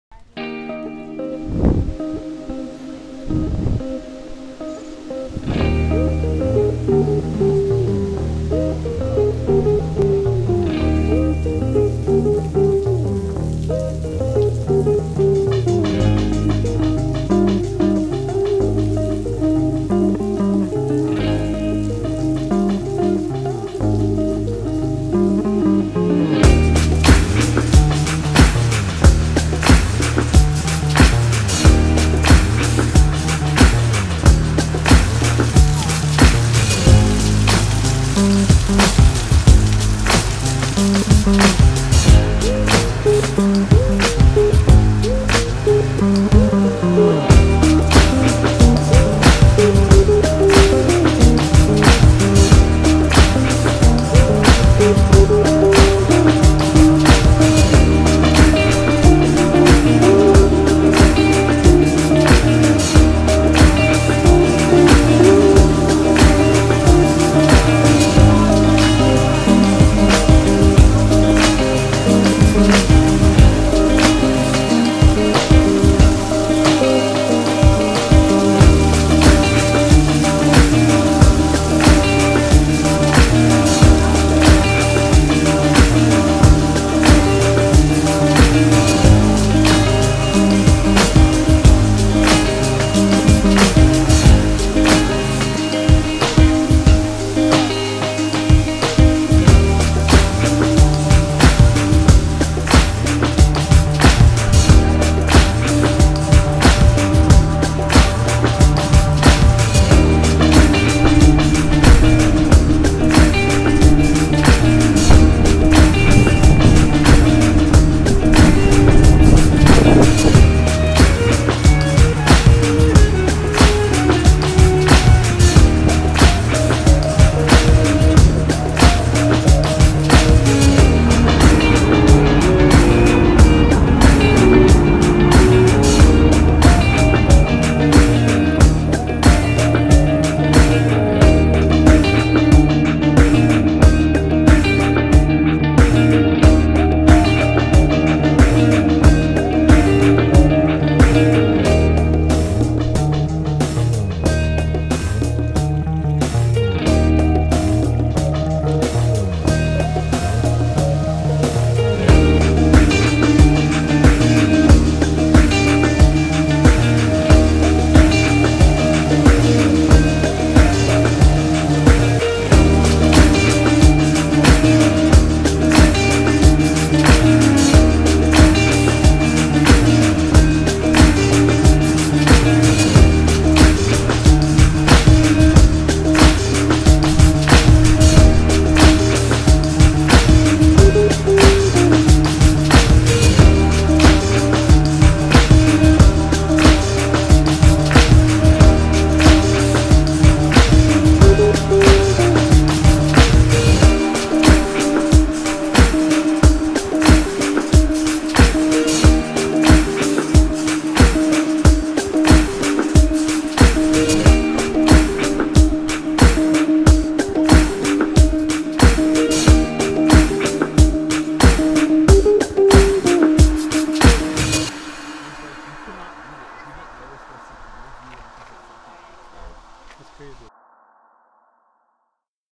Watkins Glen State Park & Taughannock Falls State Park
Took a roadtrip to Watkins Glen, NY over the forth of July weekend to visit Watkins Glen State Park and Taughannock Falls State Park.  Also did a few wineries, although not featured in this video.